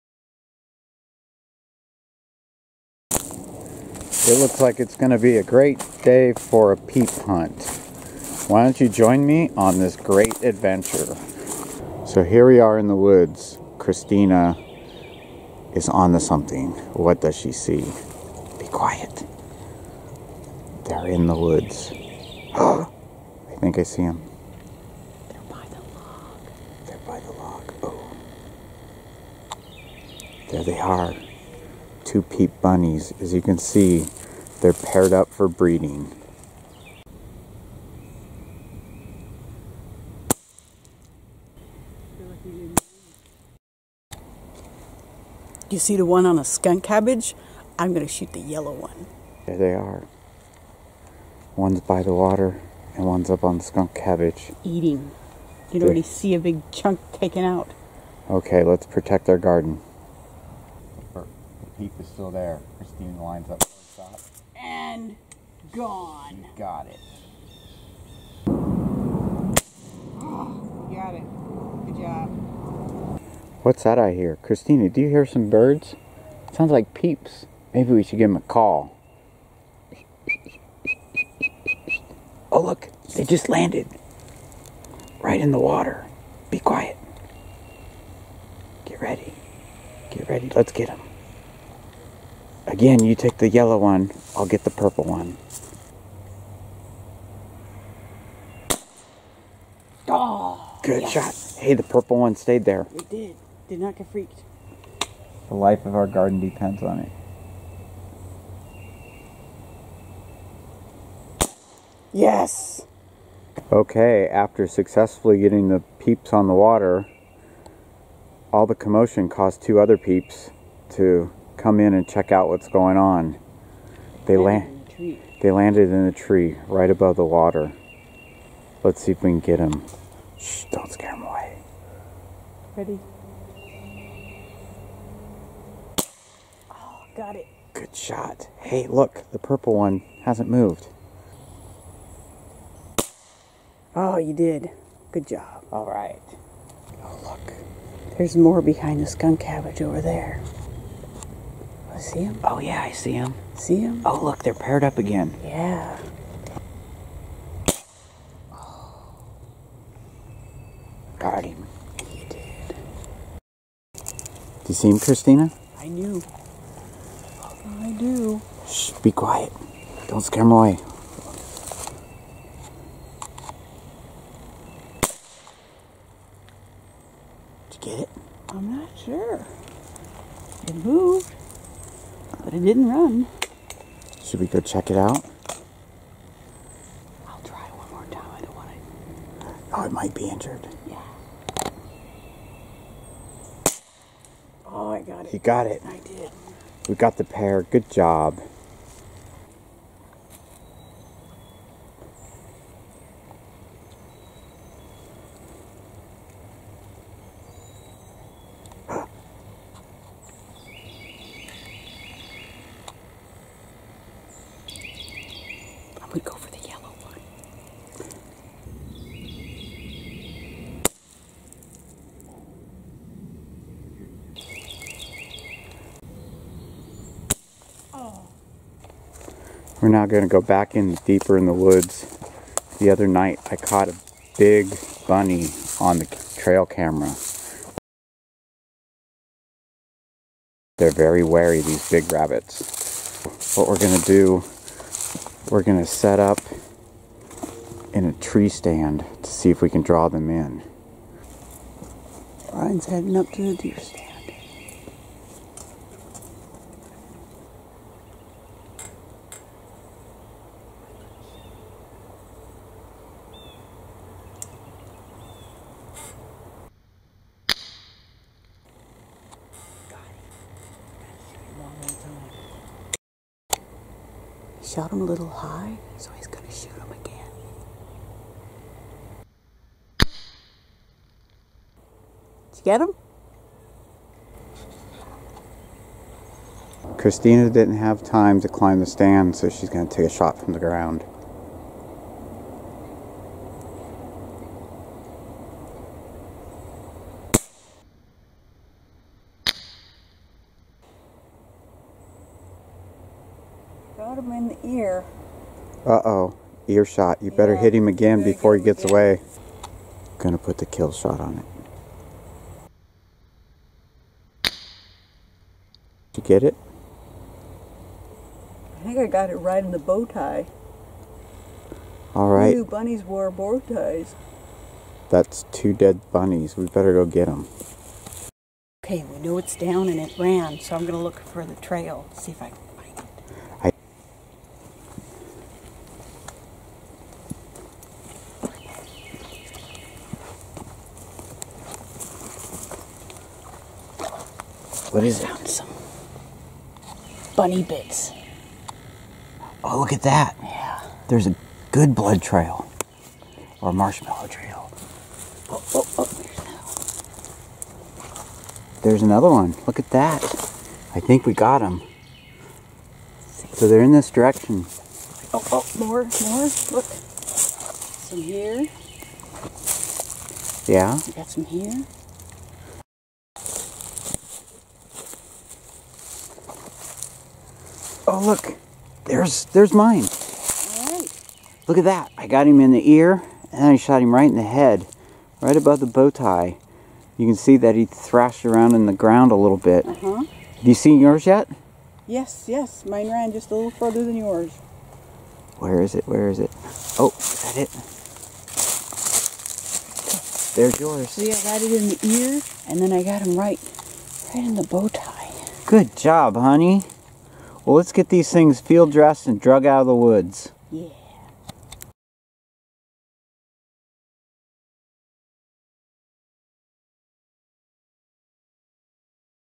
Peephunt 2017
WARNING: May not be suitable for vegans, young children, bunny huggers, or those without a sense of humor.

Every year, the bunny Peeps show up in our yard and wreck havoc with the daffodils and chase the dogs. This year we were prepared to protect our woods and keep the dogs out of the vet's office.

Since Peeps are creatures of structure, we set out into our woods to find these elusive cretures. Hint: It pays to spend time scouting before the hunt.

We were excited to see that the chick Peeps showed up after the hard south winds the night before.

Being extra careful , we set up to take out this menace. Christina  took care of the female yellow Peeps, and I went for the more docile purple phase males.   As a bonus the rare albino giant Peeps showed up at our feeder.

As you can see, we were successful and enjoyed a nice meal of free range and gluten free Peeps.

Can't wait until next Peep season.